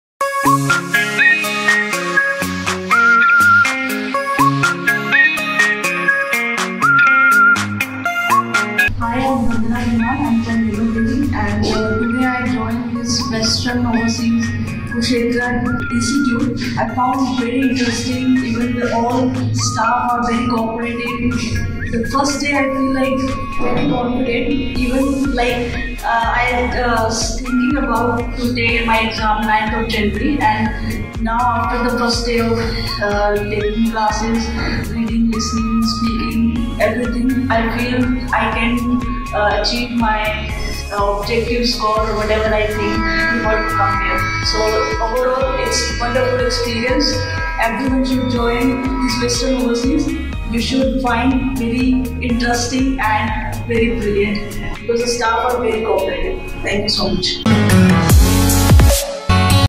Student feedback after attending IELTS Classes | First Day Review
Students feedback after attending IELTS Regular Classes

First Day-First Review. Thanks to Ms. Vandana for her valuable feedback. The student is from Nilokheri, Karnal.

According to her all the trainers of WesternOverseas are friendly in nature and even you can not feel any kind of hesitation.

Join IELTS-PTE Online and Regular Classes